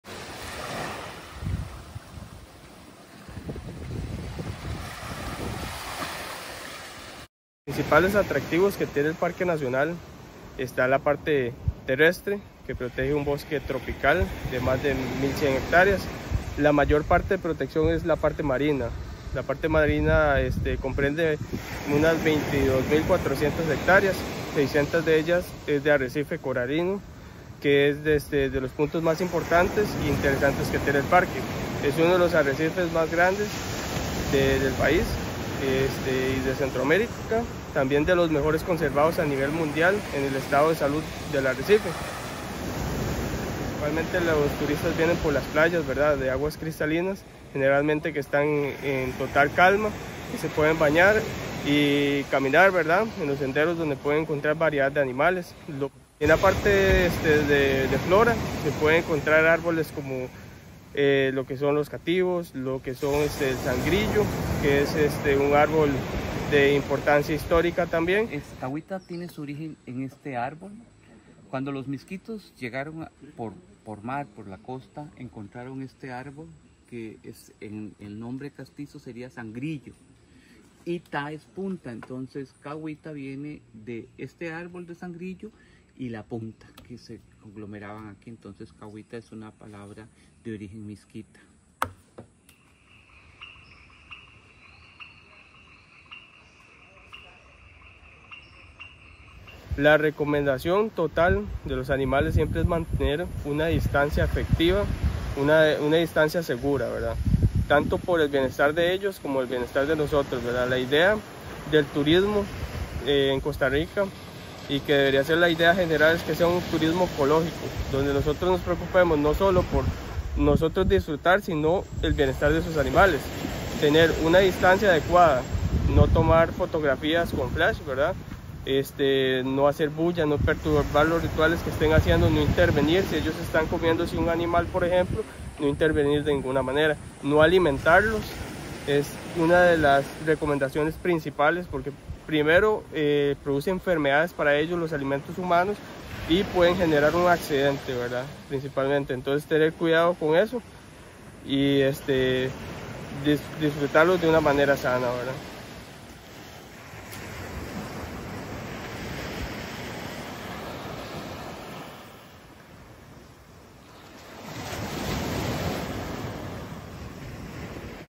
Parque Nacional Cahuita, Costa Rica
La majestuosidad del Caribe Sur de Costa Rica 🇨🇷 en el Parque Nacional Cahuita. Noviembre 2024